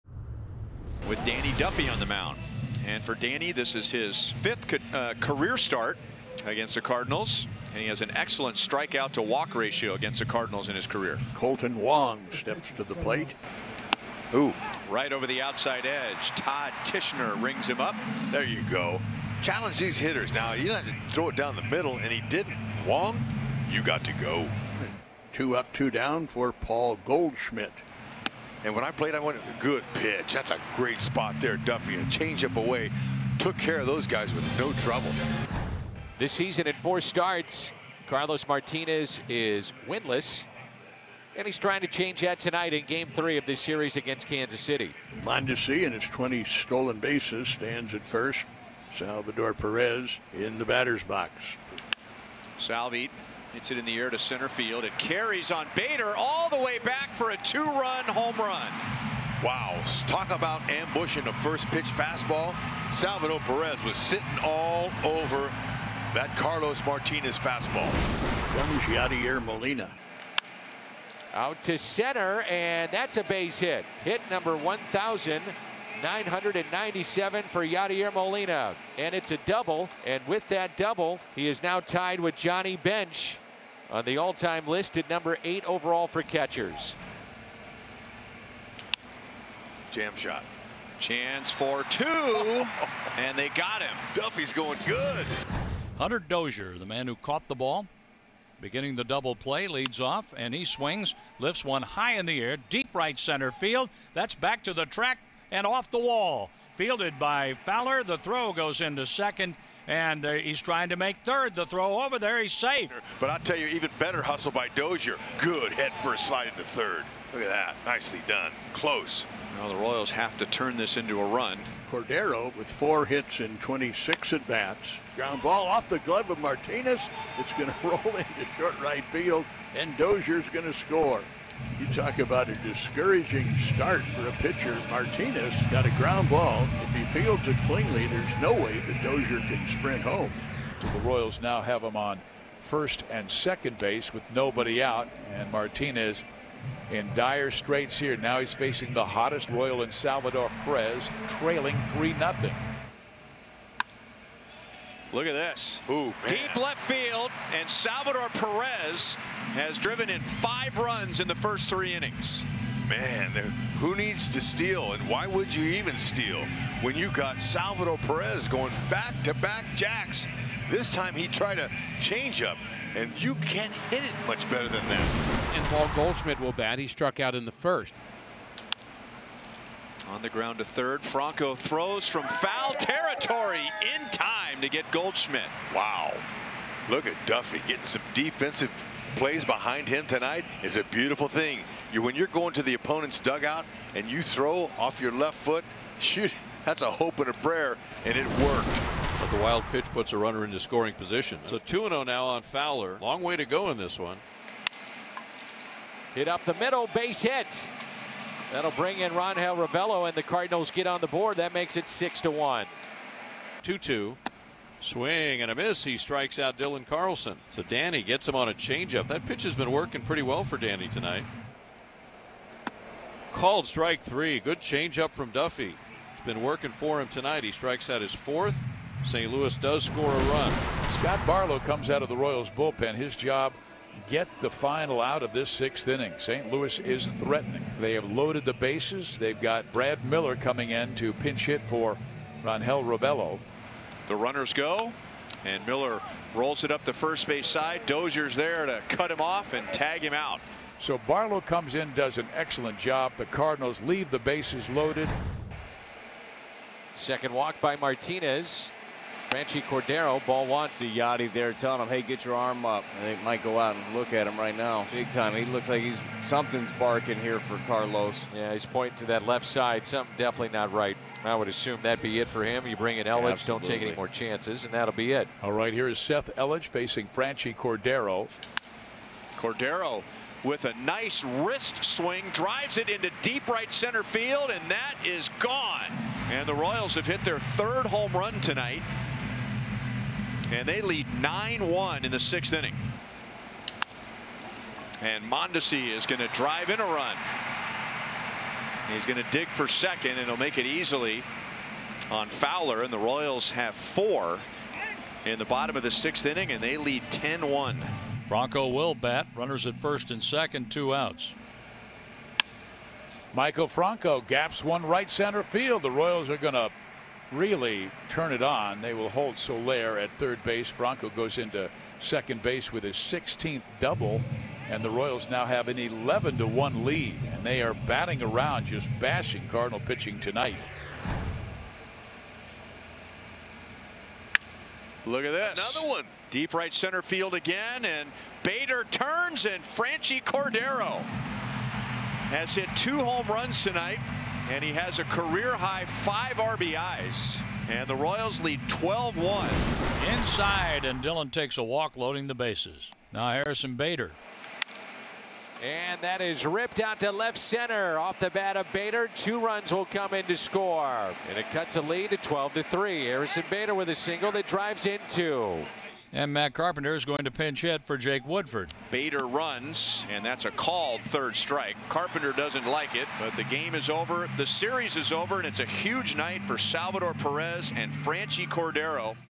Salvador Pérez, Franchy Cordero lead Royals to 12-3 win | Cardinals-Royals Game Highlights 9/23/20
Salvador Pérez and Franchy Cordero both hit two home runs to propel the Royals to a 12-3 victory over the Cardinals